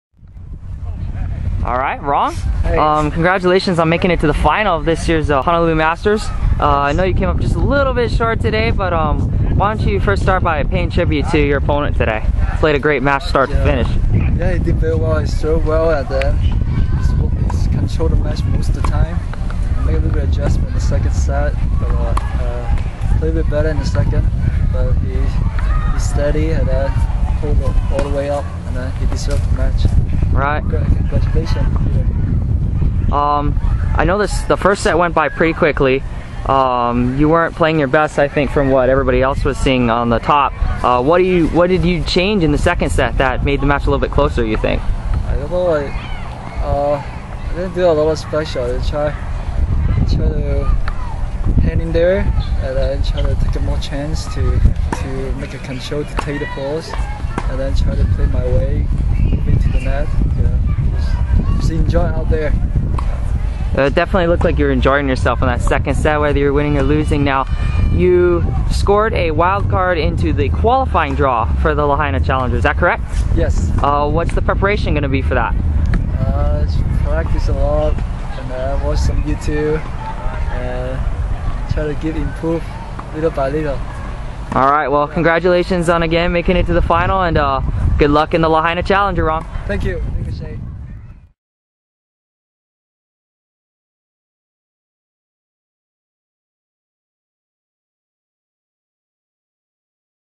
Honolulu Masters 2013: Men's Singles 2nd place Interview
The Honolulu Masters
USTA Sanctioned Event
September, 2013
The Oahu Club
Winner receives a Wildcard entry into the 2014 Royal Lahaina Challenger

Men's Singles:
Championship Match (Finals)
(1) Petr Michnev  def.  (2) Rong Ma
6-0, 6-4

Rong reflects on his effort in the finals of this year's Honolulu Masters.